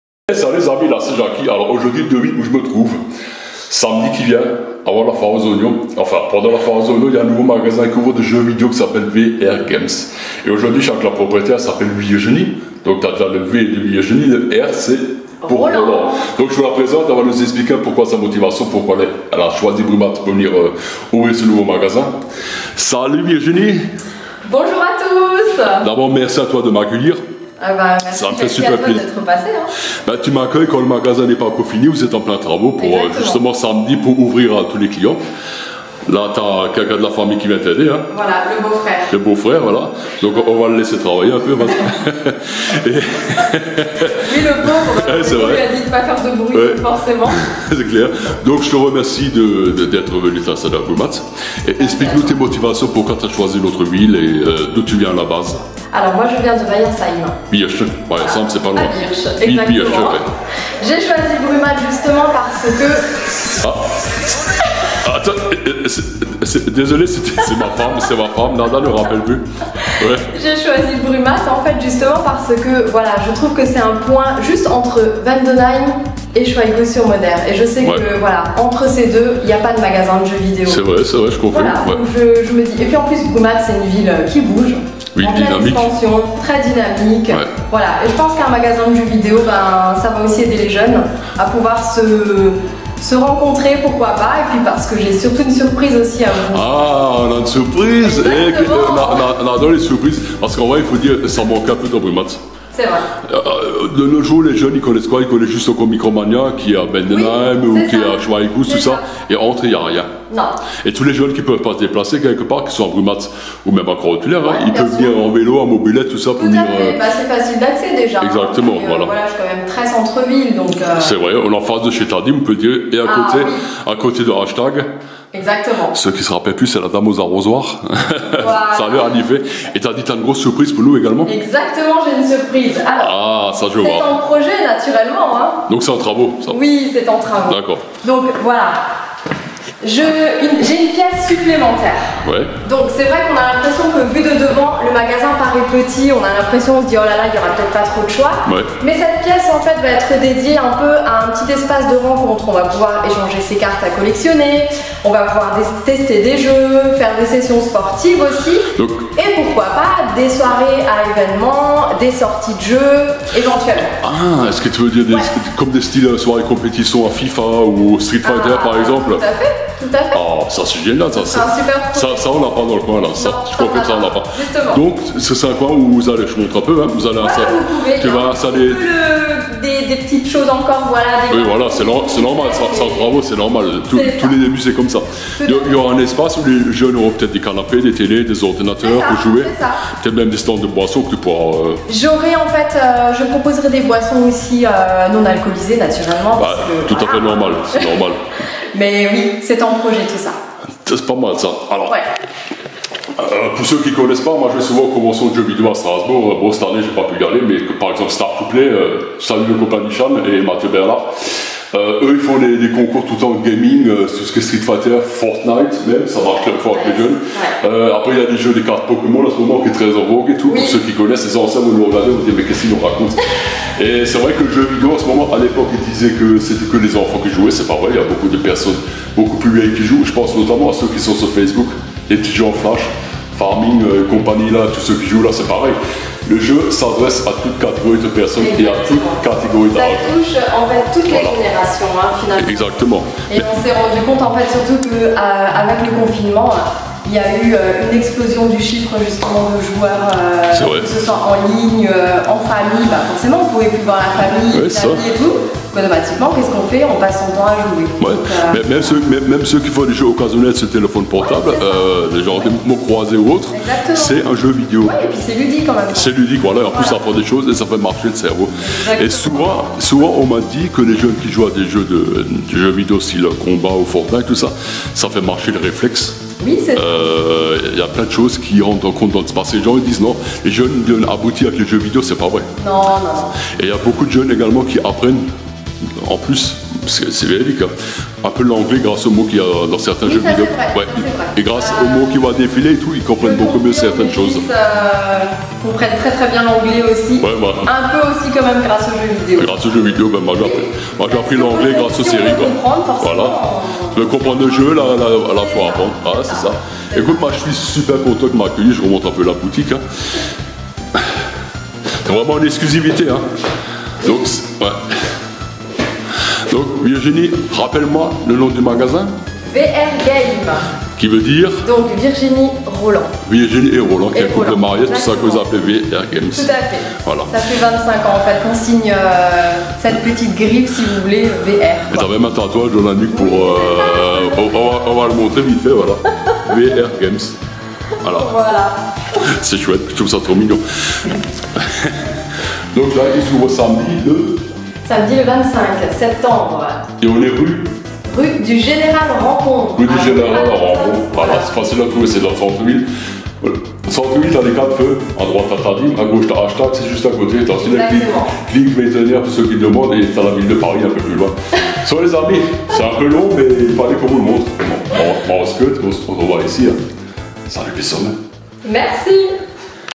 Brumath et son magasin de Jeux Vidéos VR GAME
Aujourd'hui je suis avec Virginie la gérante du magasin de Jeux Vidéos de Brumath du nom de VR GAME.
Elle est son mari Roland vont ouvrir pour notre plus grand plaisir samedi pendant la foire aux oignons.
Donc le nom VR proviens des 2 prénoms du couple Virginie et Roland.
Beaucoup de belles choses seront a vendre qui concerne la pop culture, les jeux videos, les pokemons etc , ainsi que des tournois sur les plus grands titres que les jeunes aiment jouer.
Rue du Général Rampont a Brumath